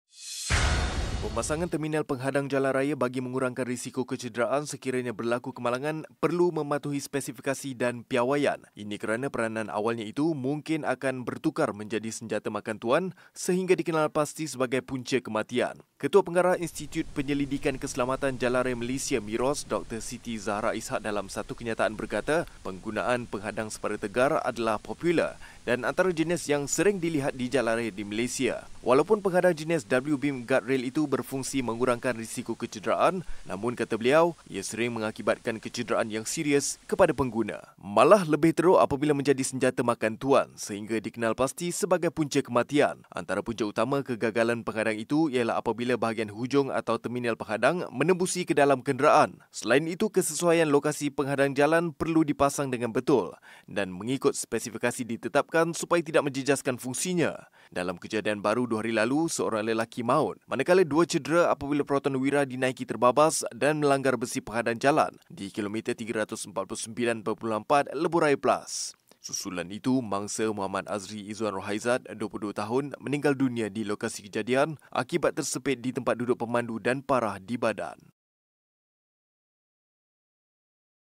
Bahaya Penghadang Jalan Tidak Patuh Piawaian
Pemasangan terminal penghadang jalan raya bagi mengurangkan risiko kecederaan sekiranya berlaku kemalangan perlu mematuhi spesifikasi dan piawaian.